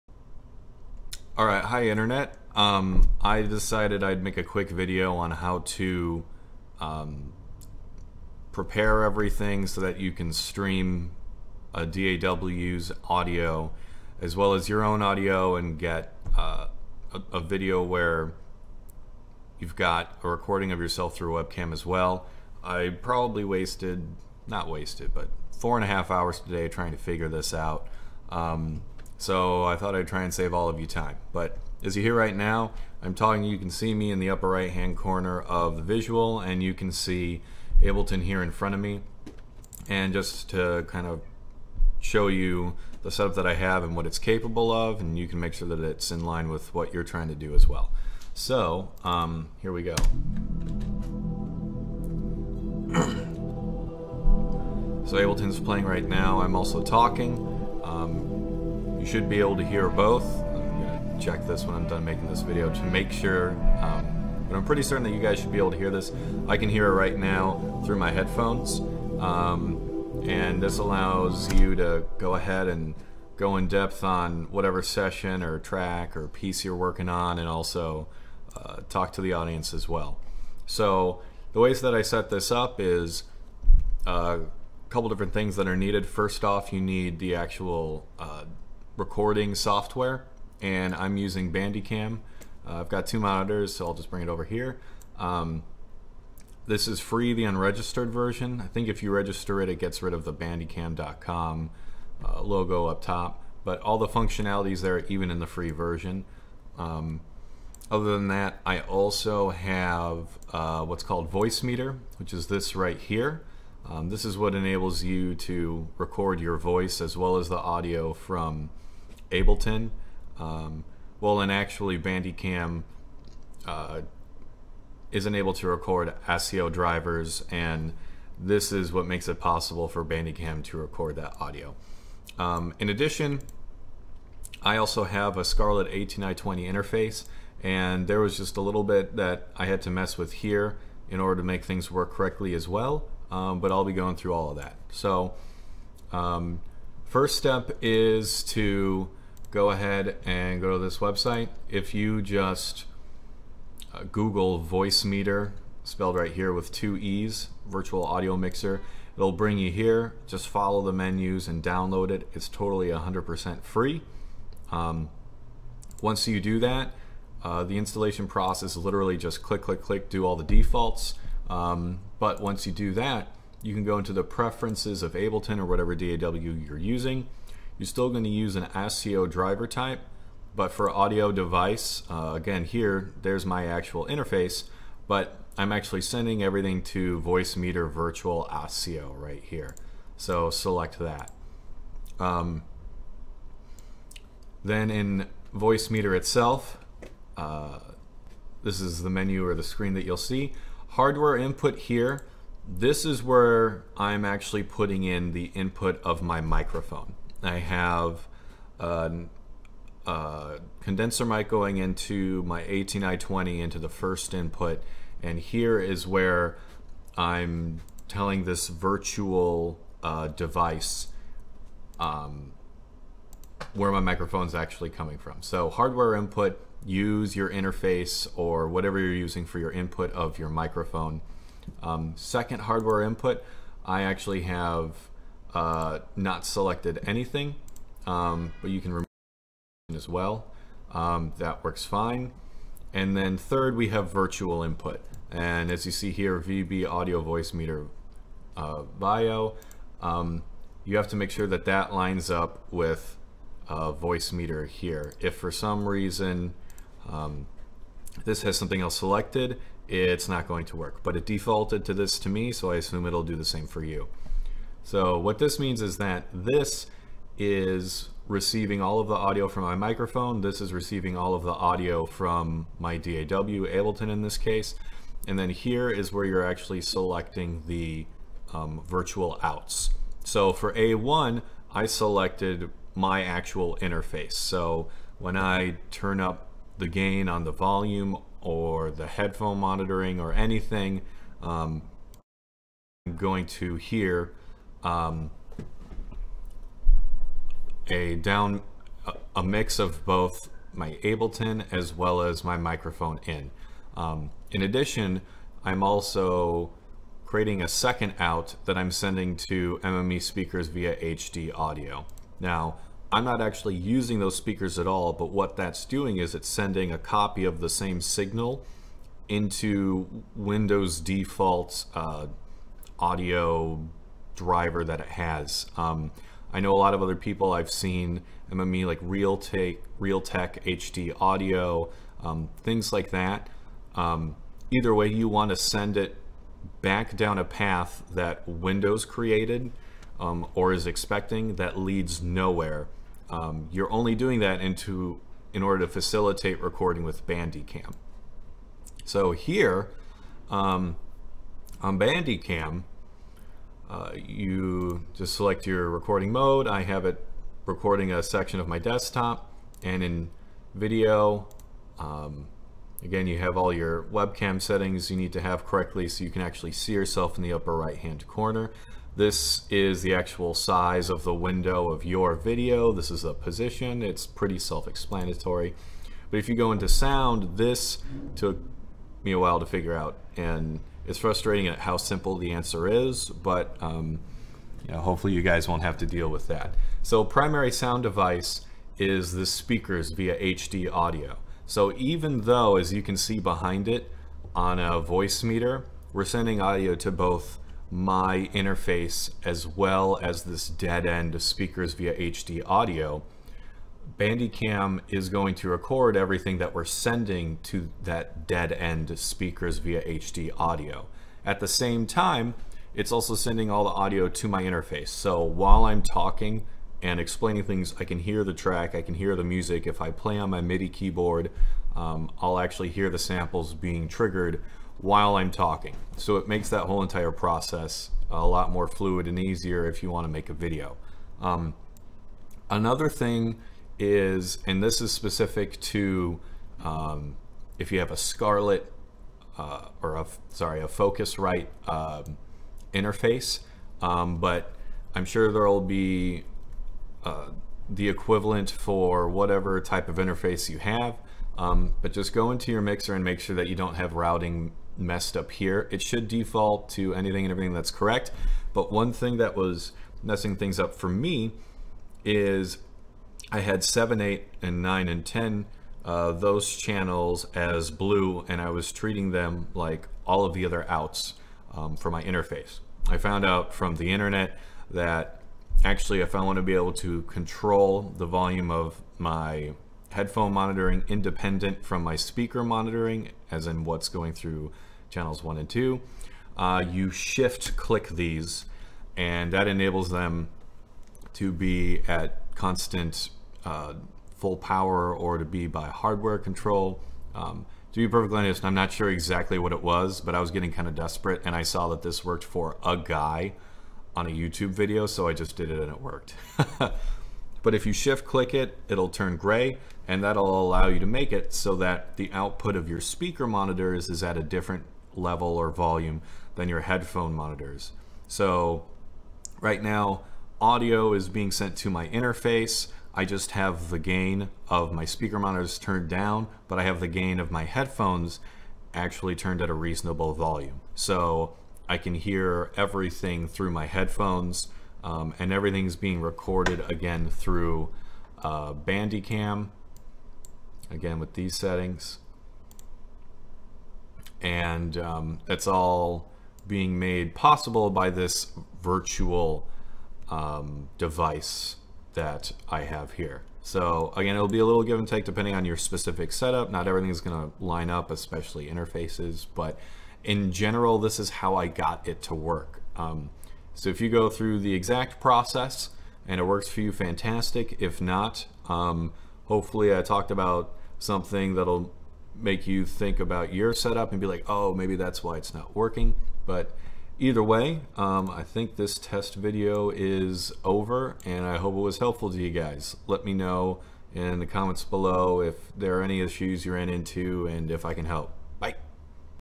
How to Screen-Record your ASIO DAW and webcam in Windows
How to Screenrecord/cast your ASIO DAW with Bandicam on Windows. Should work with any DAW and interface.